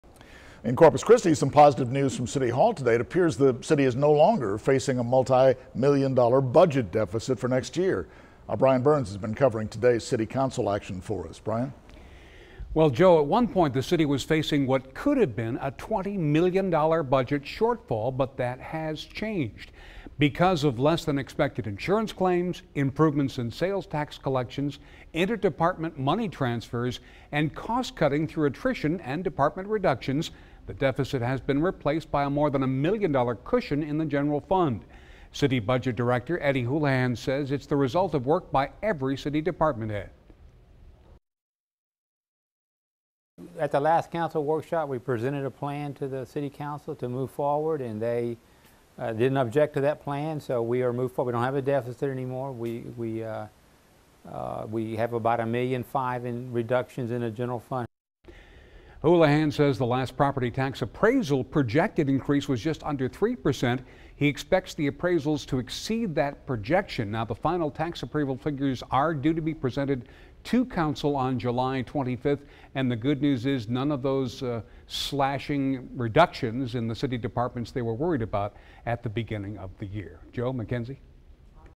City no longer facing budget shortfall
It appears the City of Corpus Christi is no longer facing a multimillion-dollar budget deficit for next year.